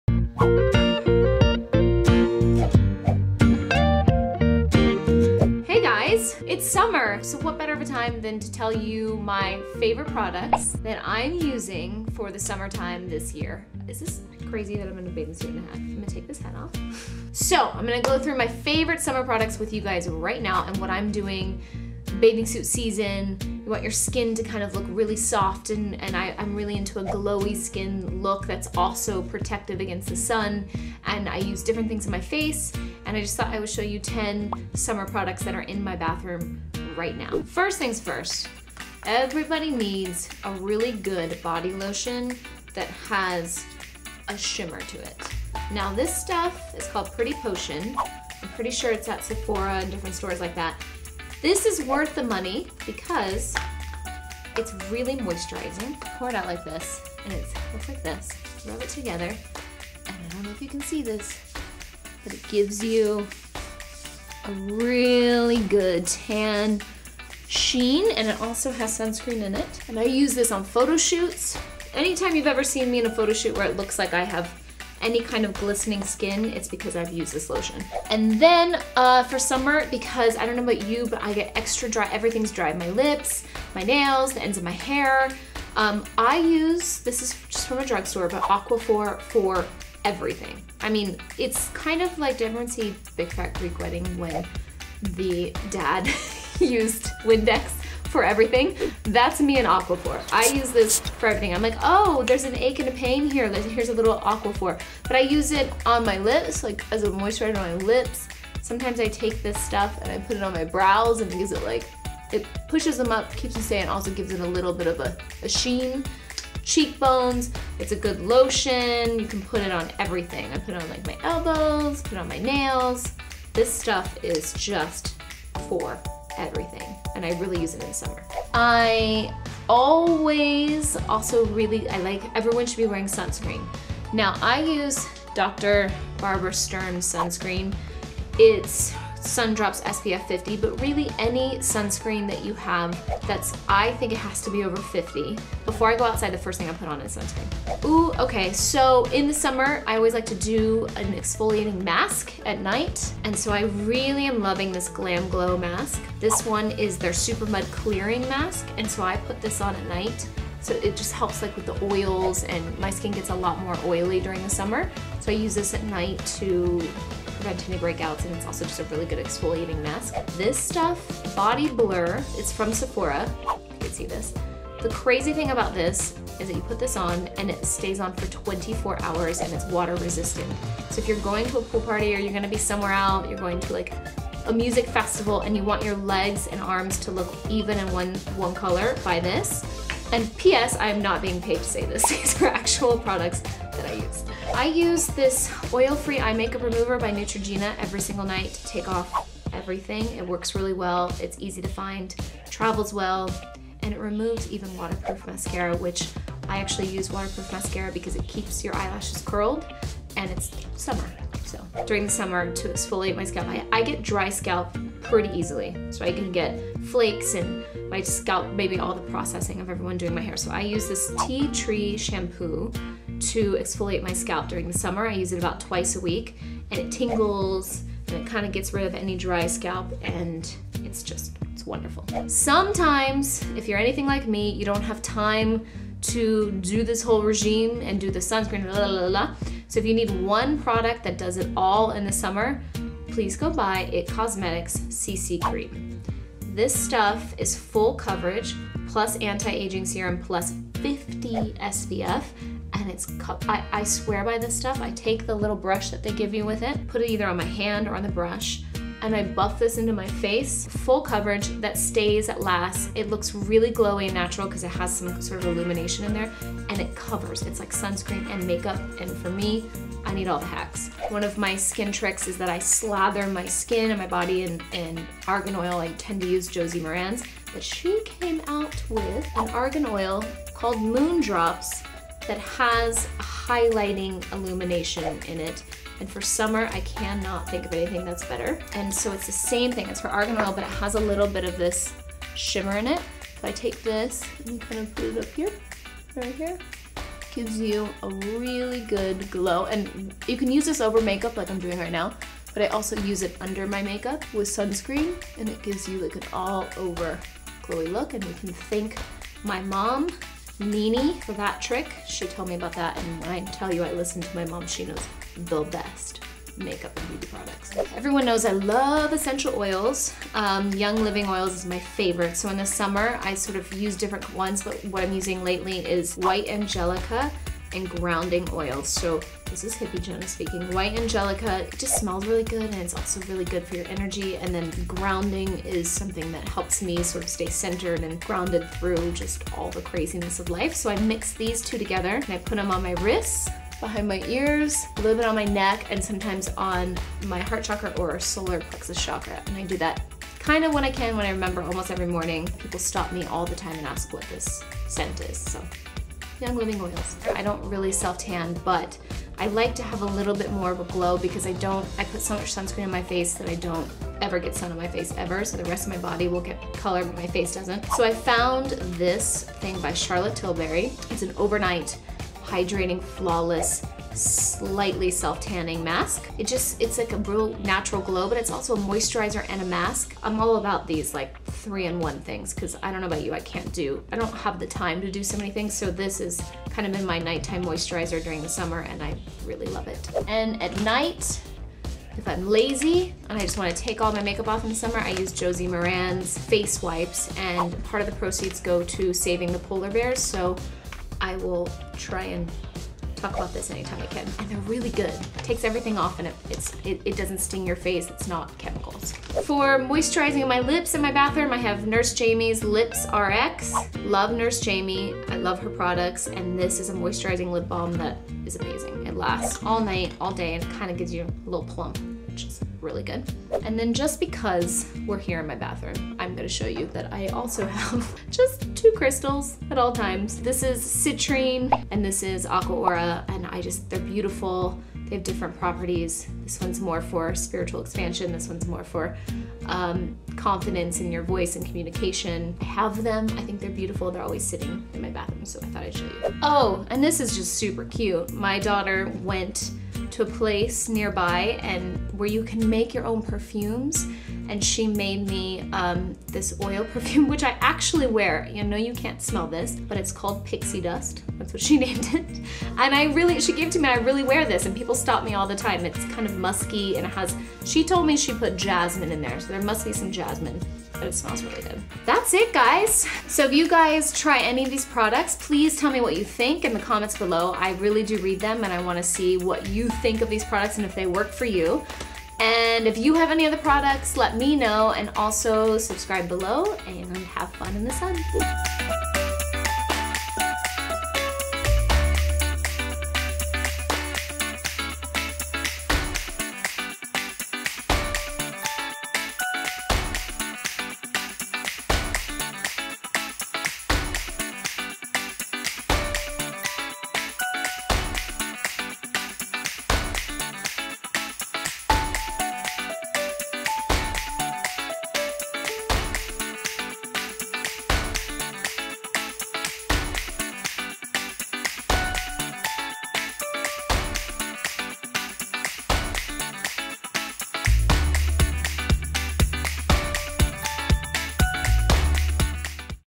My Summer Beauty Favorites | 4th of July Edition! | Jenna Dewan
Who is excited for fourth of July guys!?! Summer is in full swing! Just in time for the holiday, I am getting you ready for every single pool and beach day with my favorite summertime must haves. Do not leave home without any of these items! Especially sunscreen!